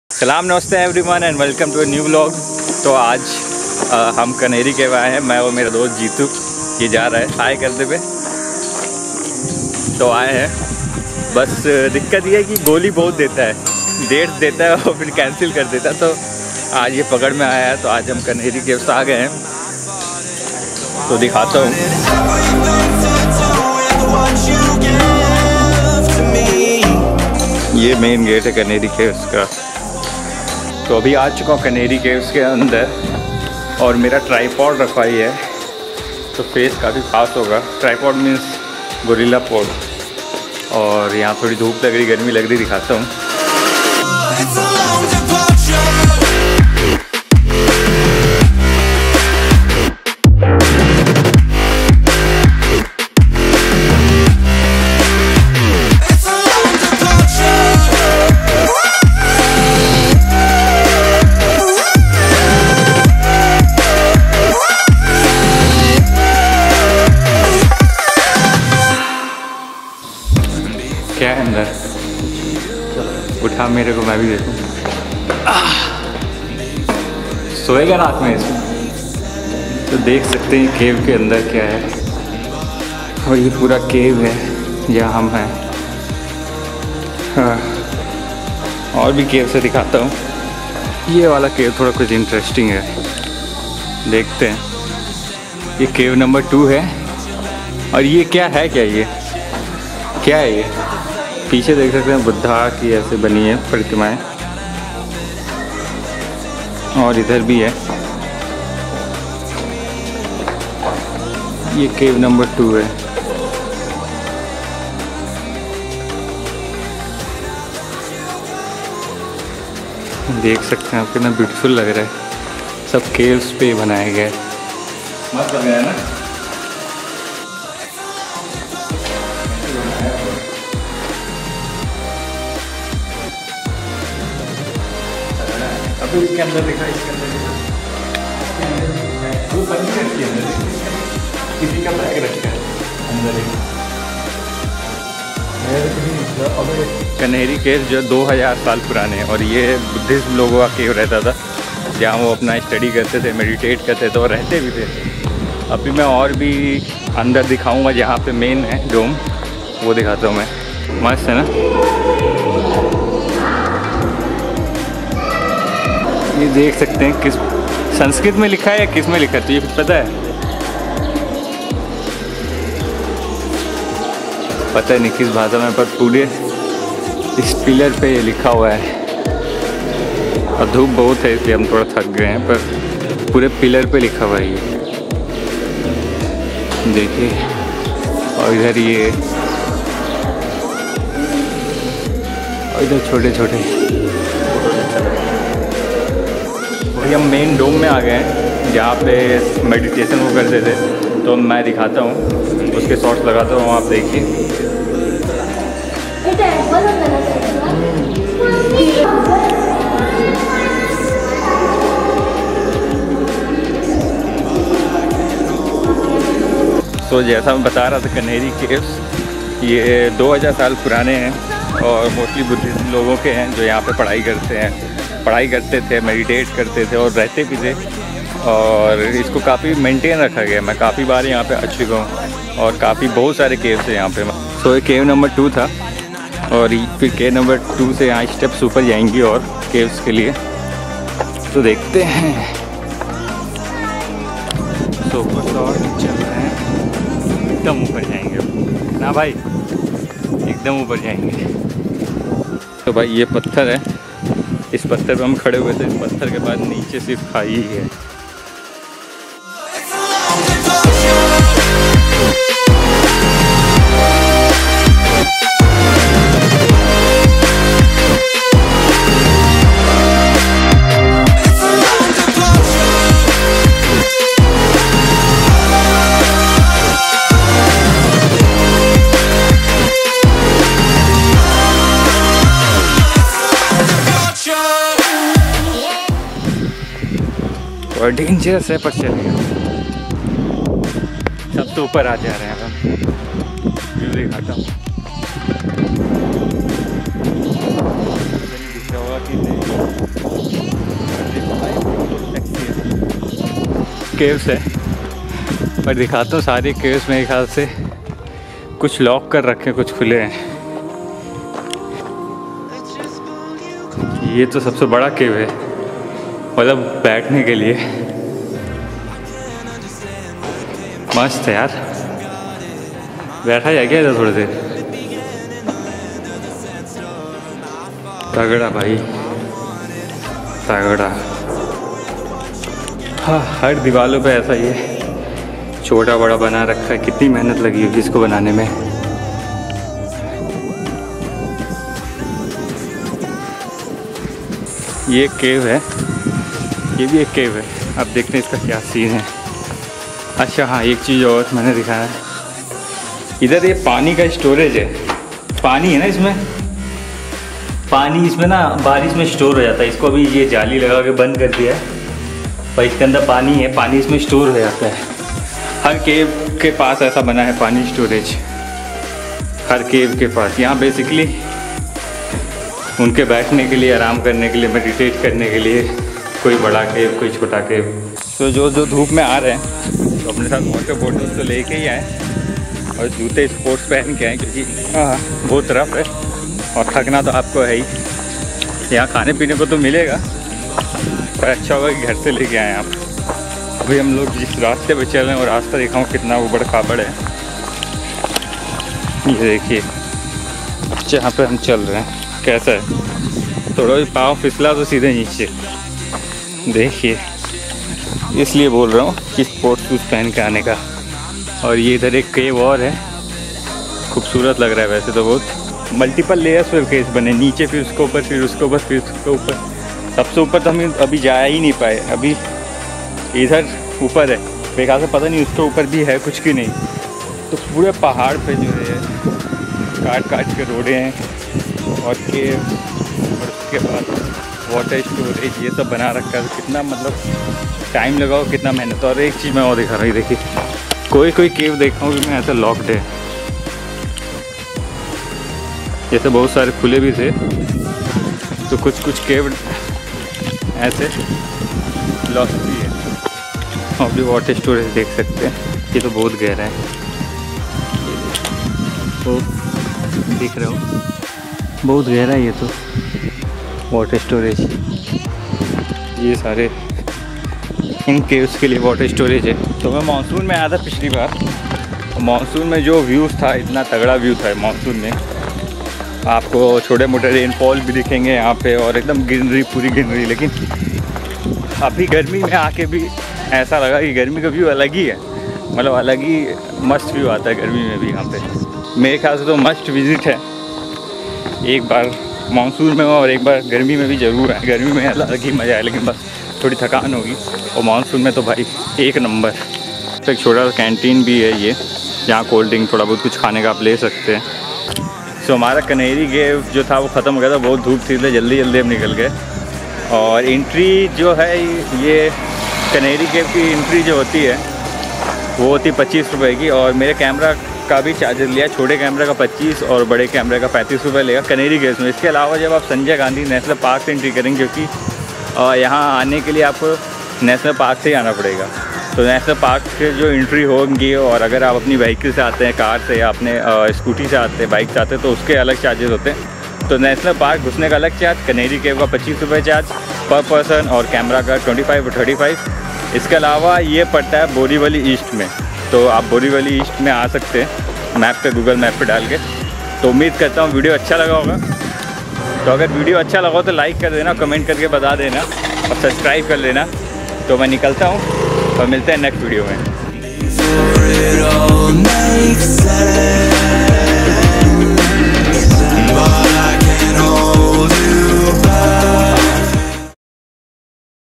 Exploring 2000 Year Old Buddhist Caves | Kanheri Caves, Mumbai Vlog
In this vlog i am exploring the 2000 year old bhuddhist caves named Kanheri Caves which is situauted in Sanjay Gandhi National Park, Mumbai.